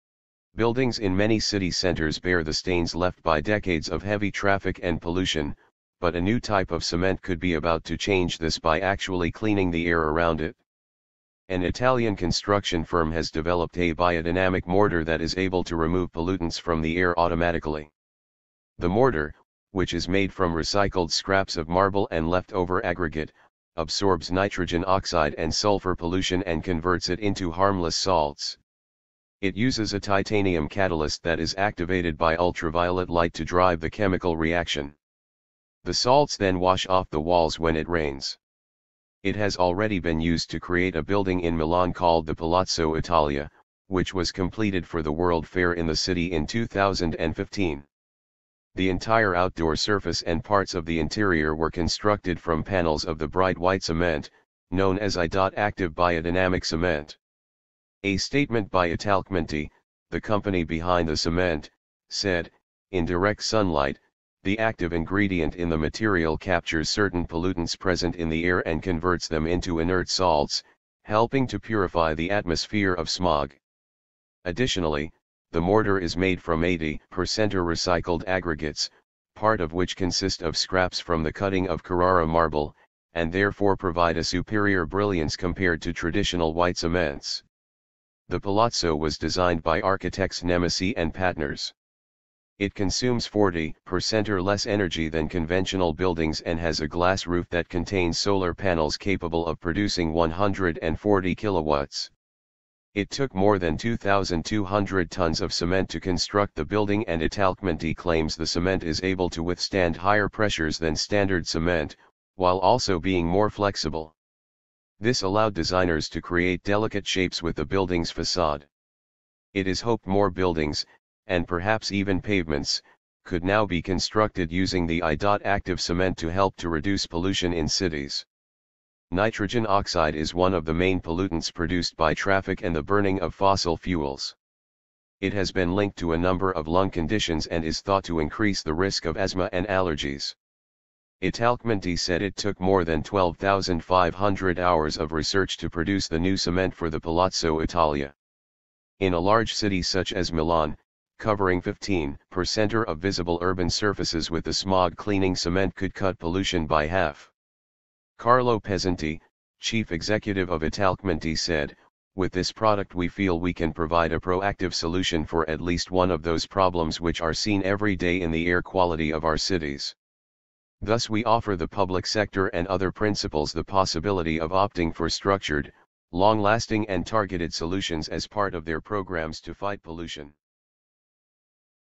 The building that 'sucks up' SMOG
The building that 'sucks up' SMOG: Cement absorbs pollution from the air and turns it into harmless salt that washes off in the rain
Buildings in many city centres bear the stains left by decades of heavy traffic and pollution, but a new type of cement could be about to change this by actually cleaning the air around it.
An Italian construction firm has developed a 'biodynamic' mortar that is able to remove pollutants from the air automatically.